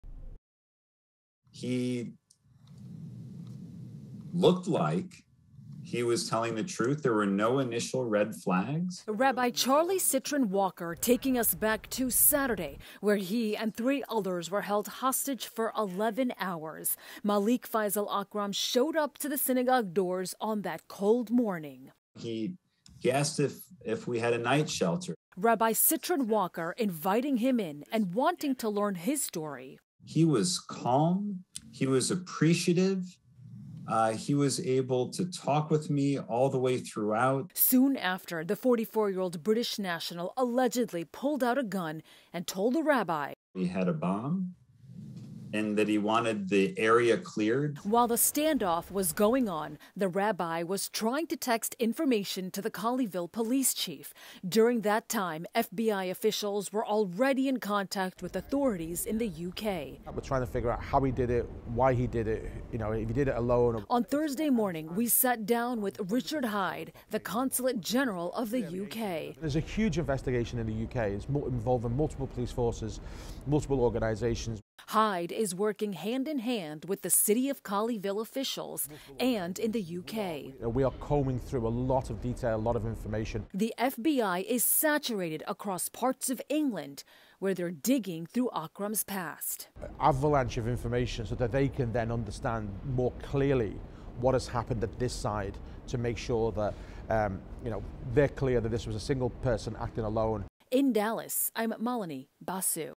2 more arrests made in connection to Colleyville hostage crisis, motive remains under investigation
There is a huge investigation in the UK, with multiple police forces and organizations.